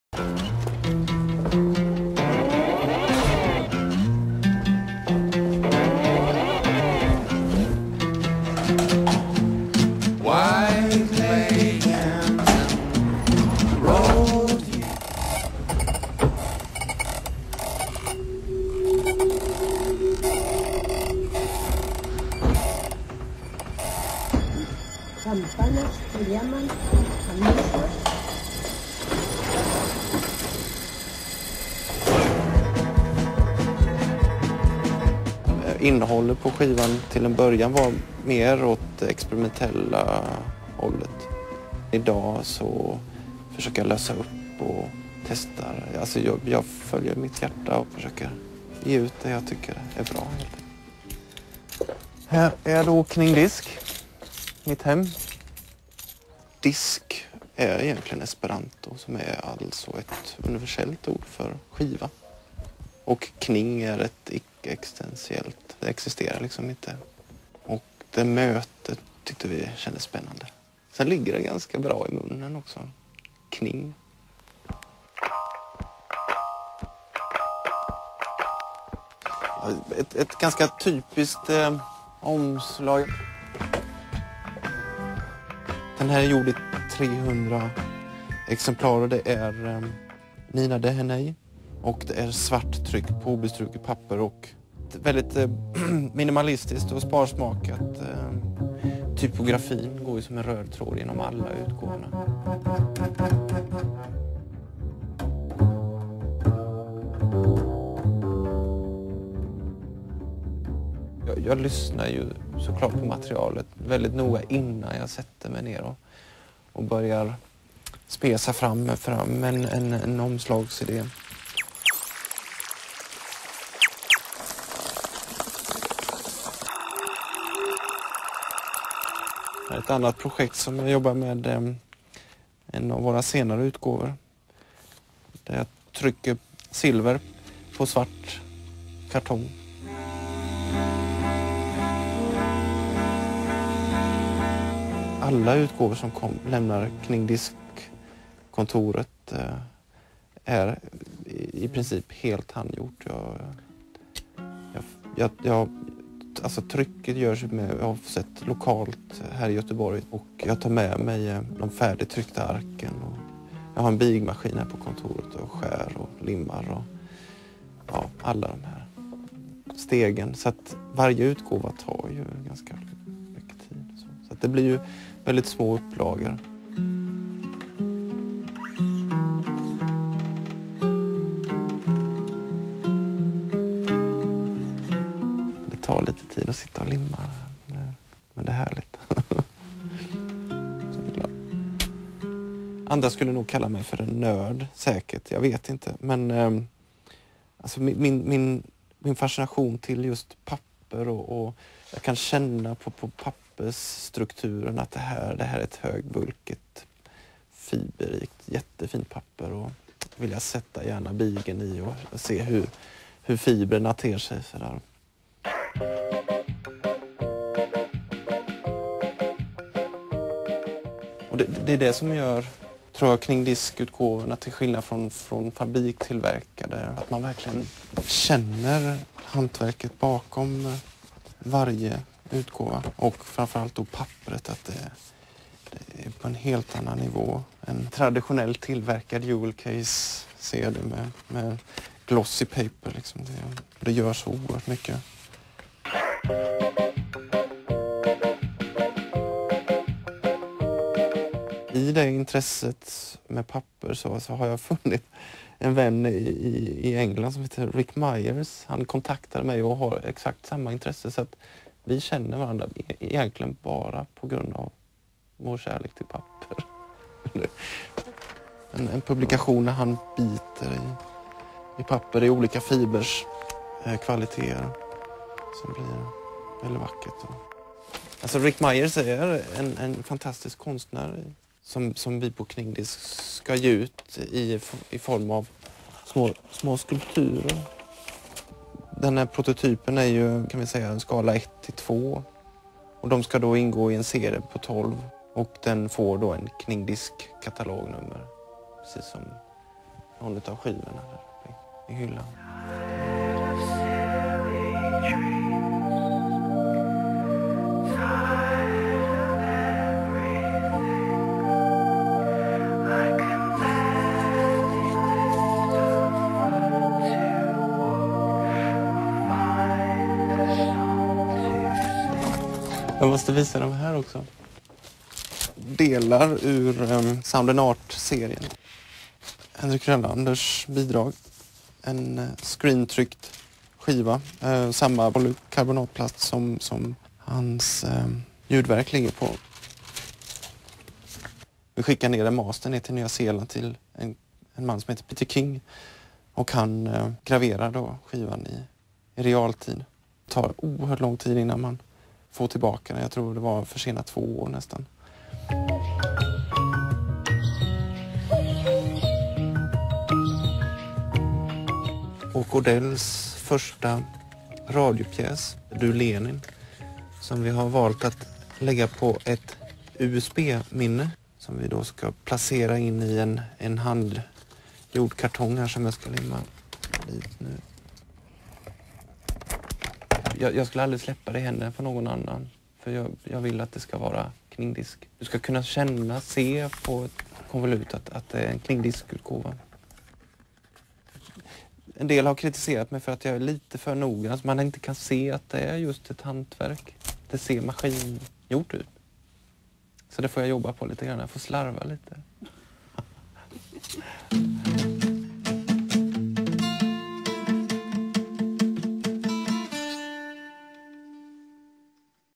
KNING DISK ON CULTURE PROGRAM SVERIGE!
KNING DISK REPORT BROADCASTED IN MARCH 2008 ON NATIONAL SWEDISH TELEVISION!